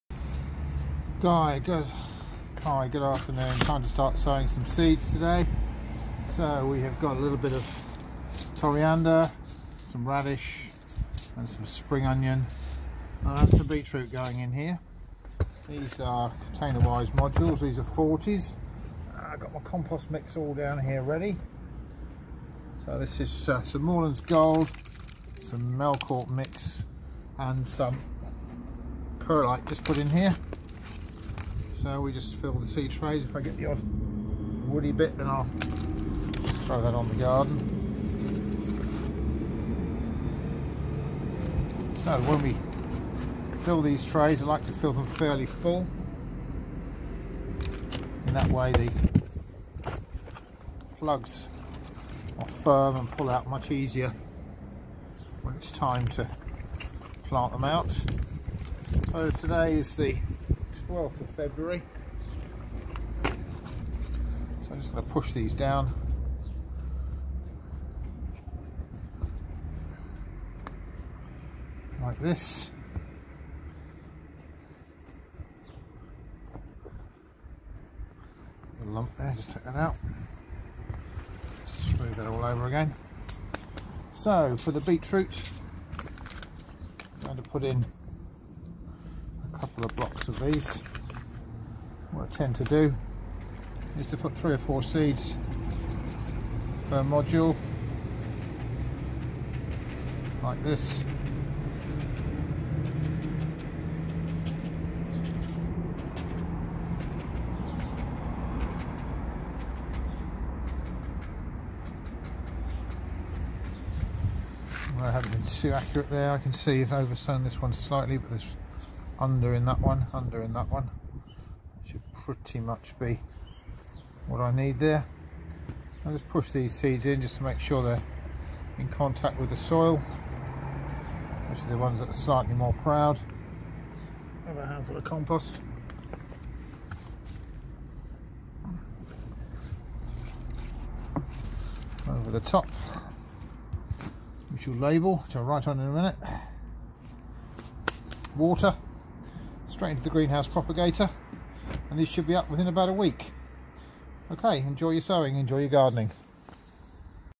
First sowings February 12th
Module sowings for the propagator.